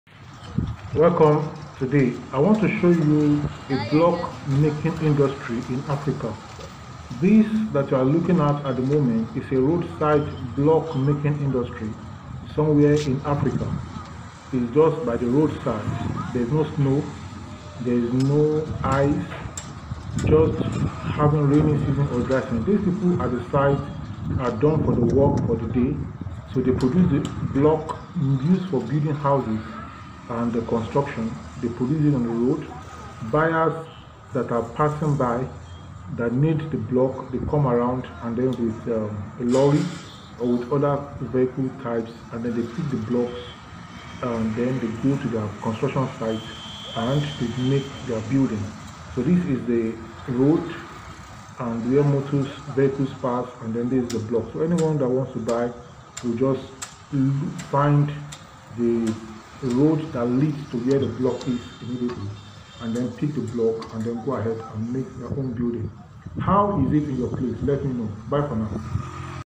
Road Side Block Making Industry In Africa (Nigeria)
Help my huzzle please.

Blocks made in Africa are used to build almost all African houses. Most of the blocks used for building are made on the roadside where customers can easily see the building block makers and stop by to pick the blocks.

In this video today, I simply show you a small roadside block-making factory with a few workers still on site.

See the video and compare it with how it's done in your location.
Tell me the difference in the comment section below.